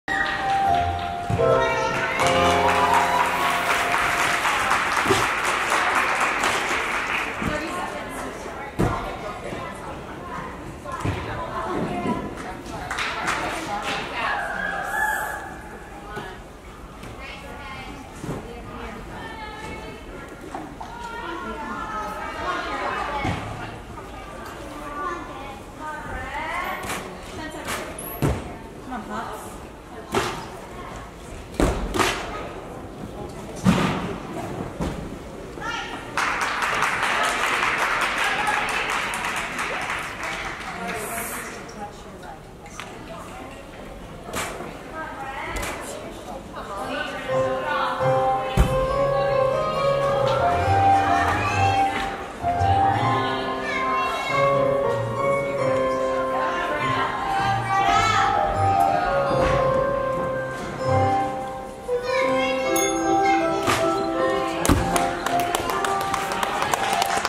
Molly Osborne 1st Beam Brown's Las Vegas 2017 Wildfire Gymnast Level 5
1st Place Beam 9.600. 1st AA 37.350.